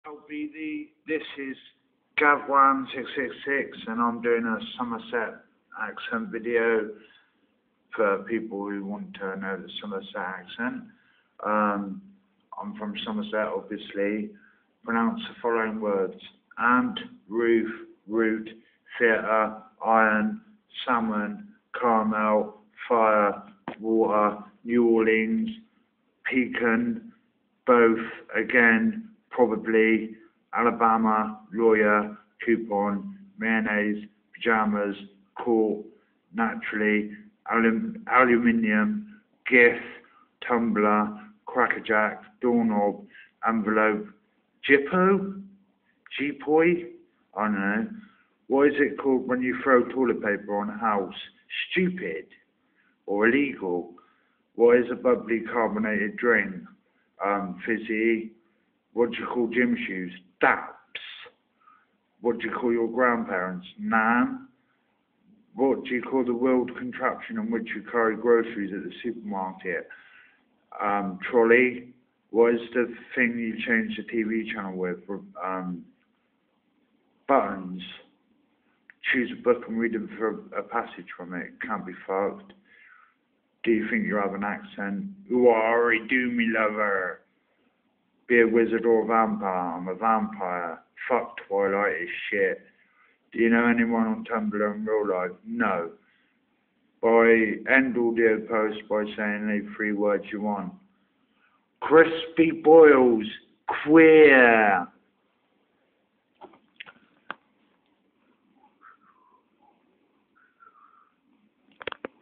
somerset england accent ooohhh arr me lover
Your name and username.
Where you're from.
Pronounce the following words: Aunt, Roof, Route, Theater, Iron, Salmon, Caramel, Fire, Water, New Orleans, Pecan, Both, Again, Probably, Alabama, Lawyer, Coupon, Mayonnaise, Pajamas, Caught, Naturally, Aluminium, GIF, Tumblr, Crackerjack, Doorknob, Envelope, GPOY.
What is a bubbly carbonated drink called?
Choose a book and read a passage from it.
Do you think you have an accent?
Be a wizard or a vampire?
Do you know anyone on Tumblr in real life?
End audio post by saying any THREE words you want.